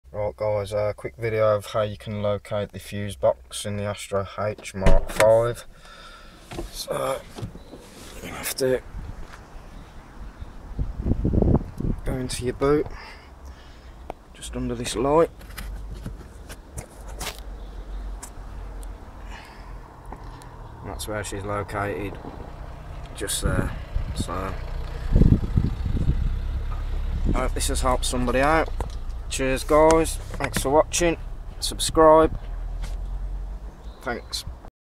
Astra H Mk5 FUSE BOX LOCATION
How to locate the fuse box in the Astra h mk5. Located in the boot on the mark 5 model instead of in the cabin itself